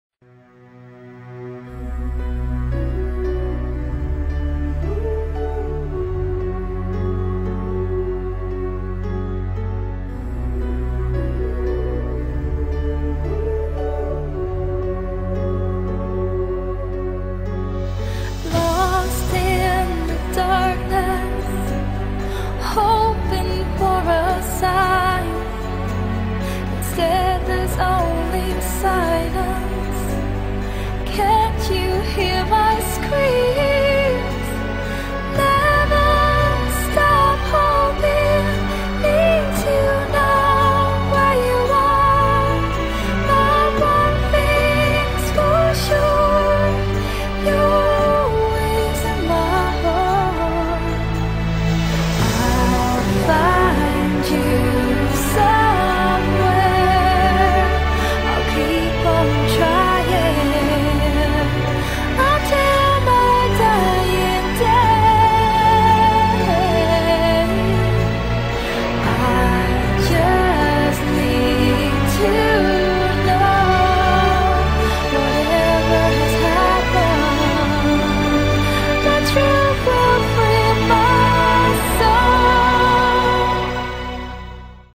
lucario dark aura ending.wmv
i figured that this songs fits the seires since lucario will try to find zoroark don't worry kara is going with him she does'nt want him to get hurt i own nothing kara belongs to me